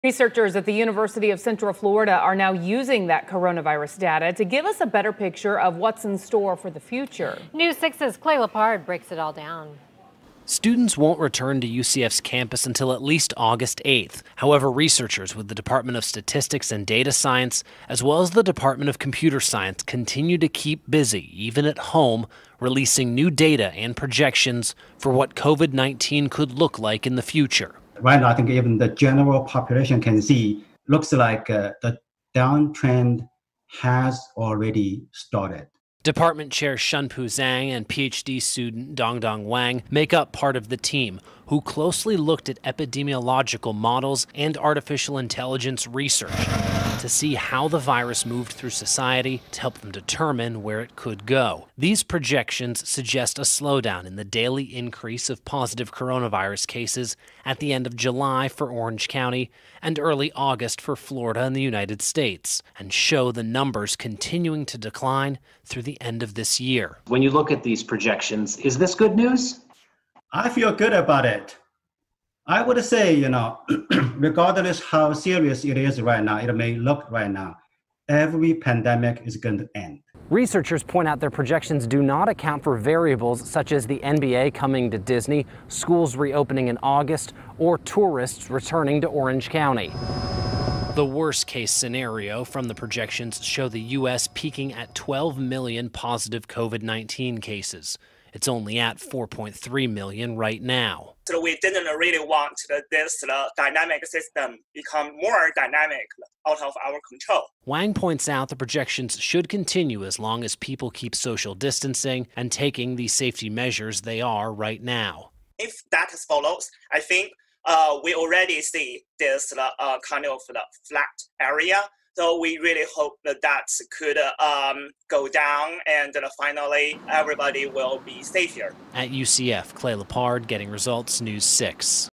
UCF projections show new COVID-19 infections could soon begin to decrease
Models developed by researchers at the University of Central Florida show that new COVID-19 cases could begin to slow down locally by August but all in all, Orange County is predicted to experience a maximum infection rate of 65,000.